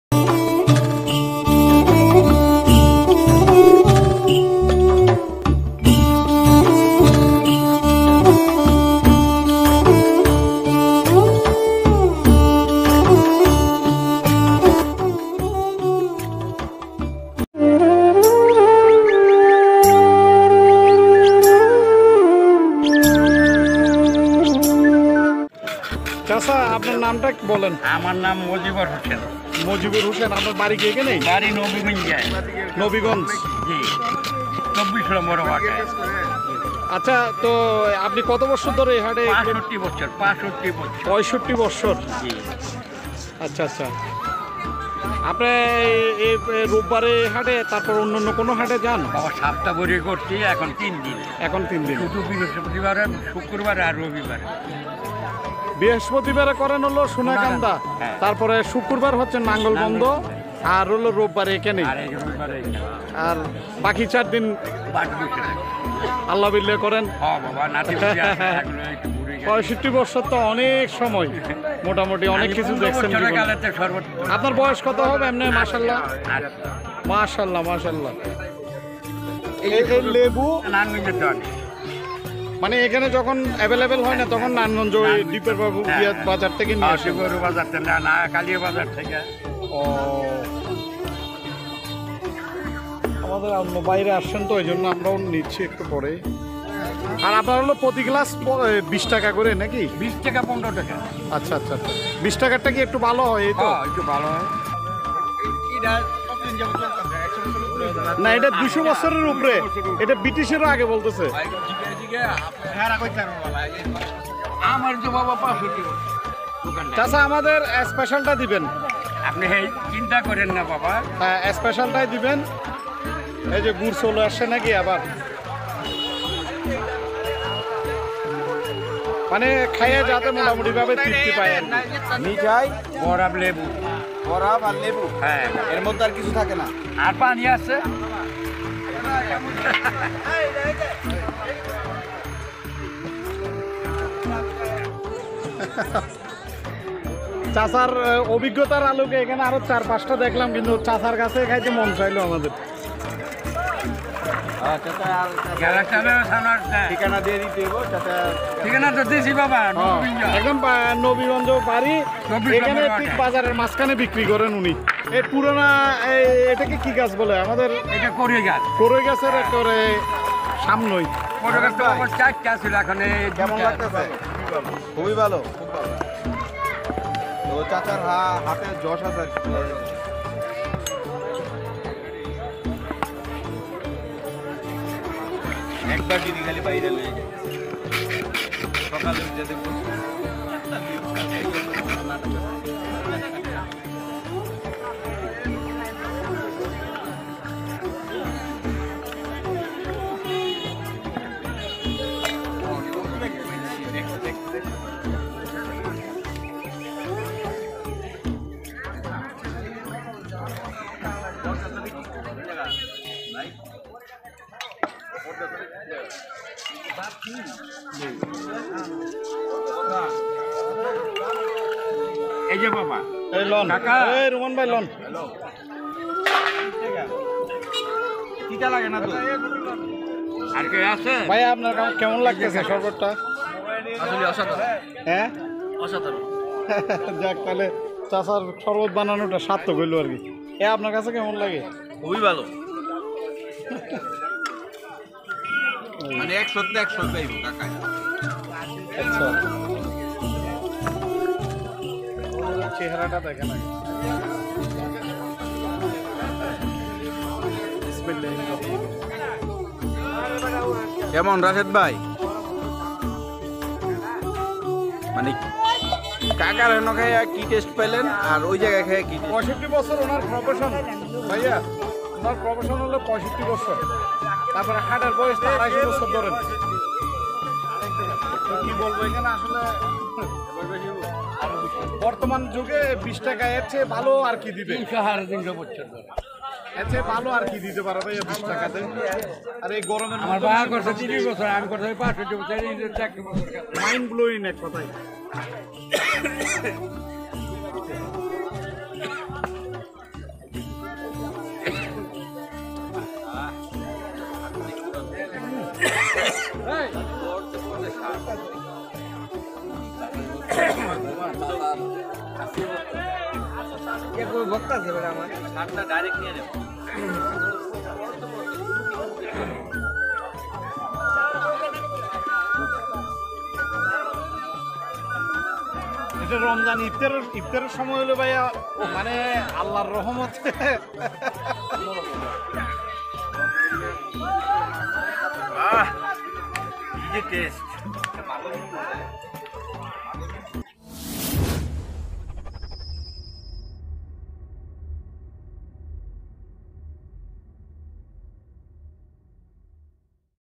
বিখ্যাত গুরের শরবত এই গরমে ঠান্ডা ঠান্ডা খাচ্ছি, আহা দিলখোশ আরাম পাচ্ছি।।
আমরা বড় ভাইদের সাথে নারায়ণগঞ্জের কেইক্কার টেক হাটে বিখ্যাত গুরের শরবত পান করছি 19 মে, 2022
#bangladeshifoodlover## rashed ##